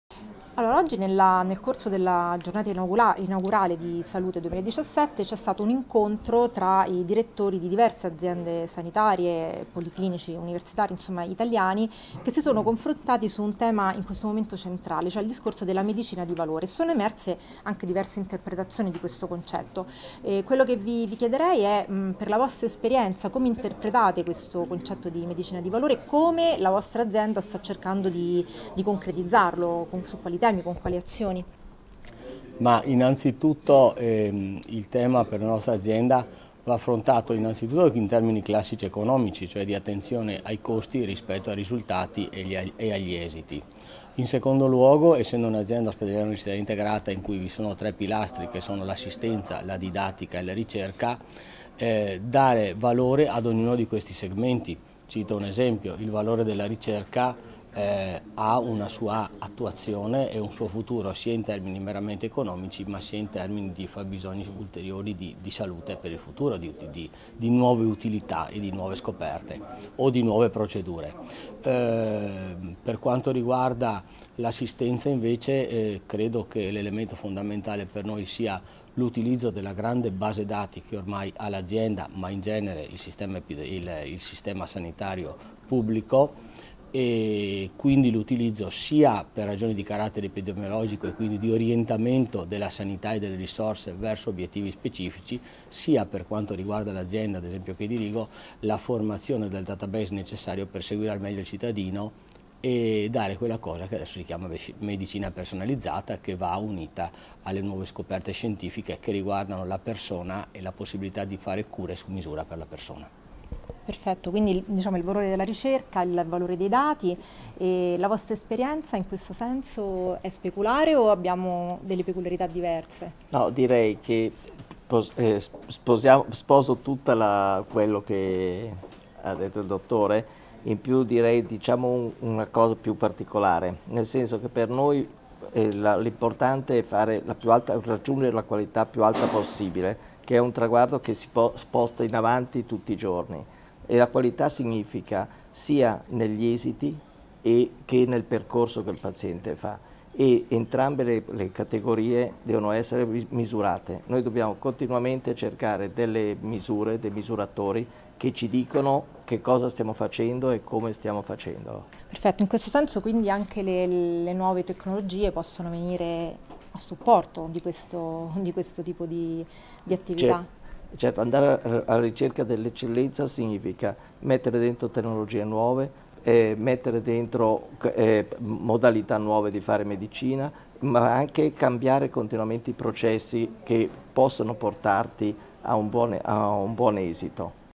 La medicina di valore tra Roma e Verona
Francesco Cobello e Norberto Silvestri, intervistati da Michela Stentella, declinano il concetto di medicina di valore così come inteso dall'Azienda Ospedaliera Universitaria Integrata Verona e dal Policlinico Universitario Campus Bio-medico di Roma.